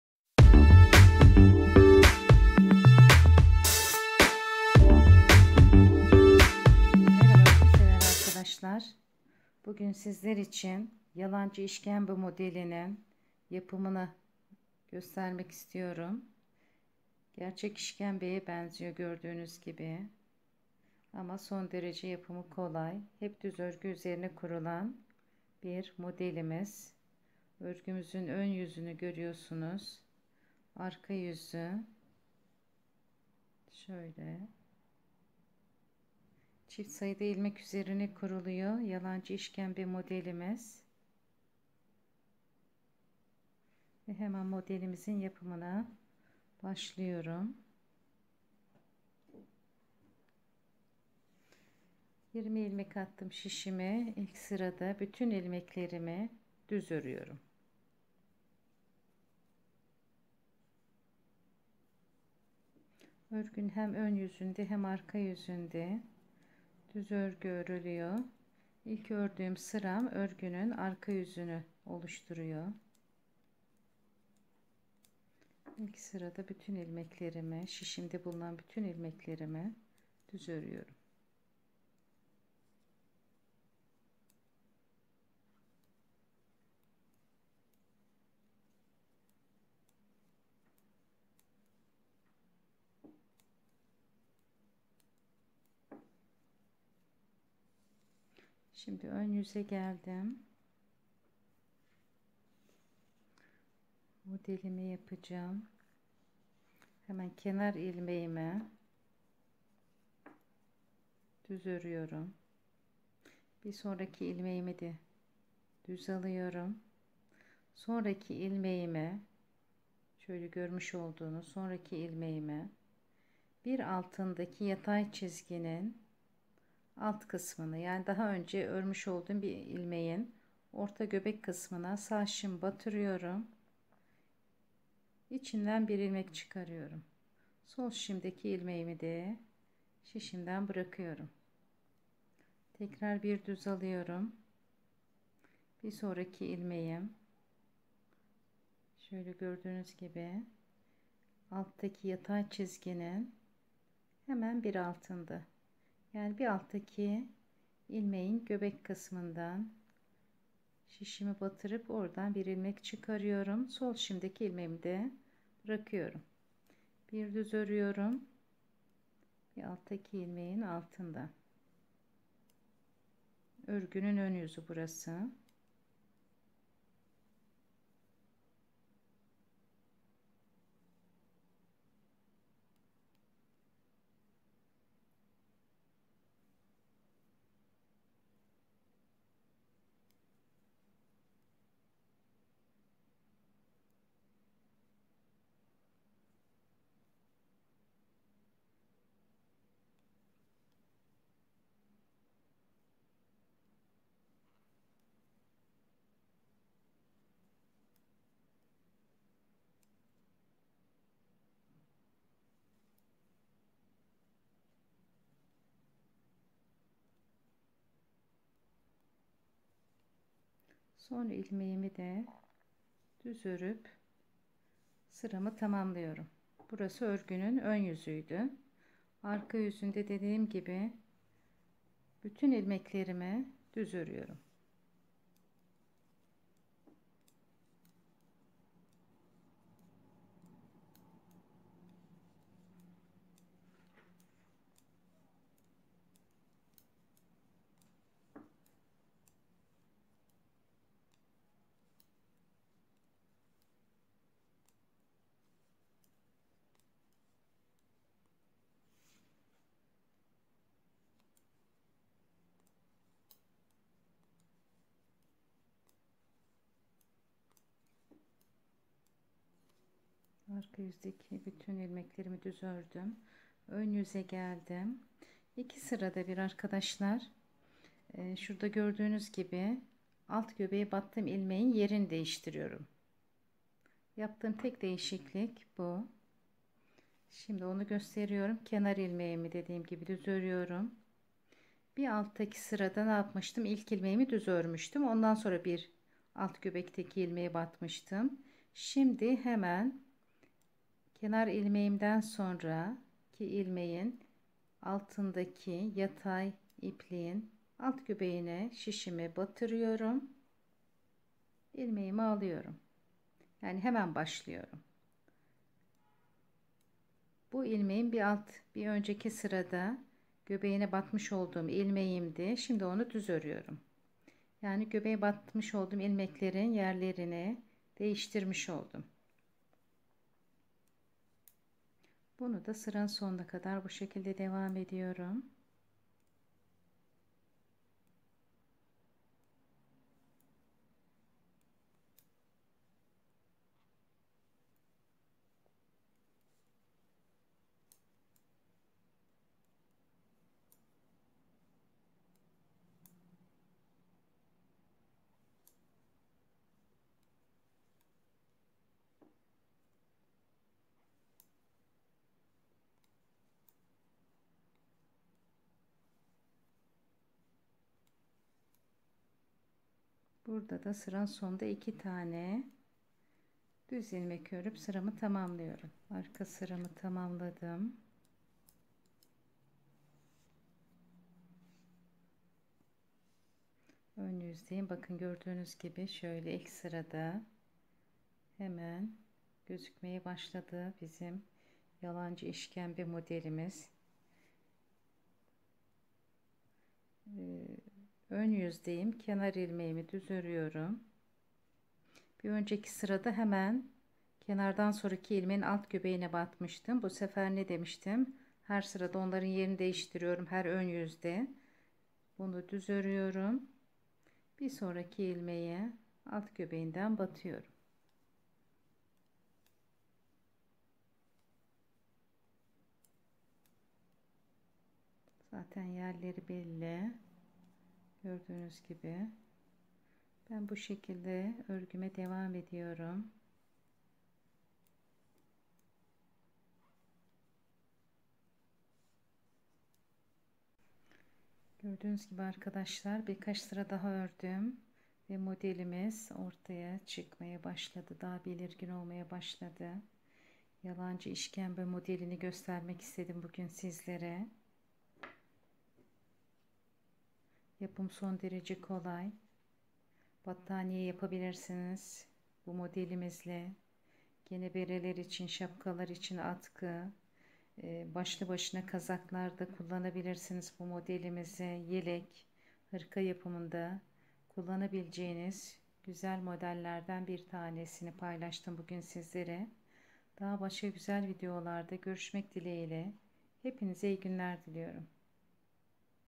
Kolay İşkembe Modeli, Nasıl Yapılır - Easy Tripe Model, How To -
örgü pattern design knitting diy kolay olan bu model, çok güzel görünümü, açıklamalı, videolu, modeli, sende yap. Handmade, el örgüsü. Gerçek işkembe modeli her zaman unutulurken,bunu rahatlıkla öreceksiniz. Model,hep düz örgü olarak örülür. yalancı işkembe modeli nasıl yapılır
 Bölümler
 0:00 görüntüsü
0:57 örgünün 1. sırası arka yüz
1:47 örgünün 2. sırası ön yüz
3:55 örgünün 3. sırası
4:47 örgünün 4. sırası
Bildirimler için zil işaretini açmayı unutmayınız.

Beni  sosyal medyadan takip edebilirsiniz.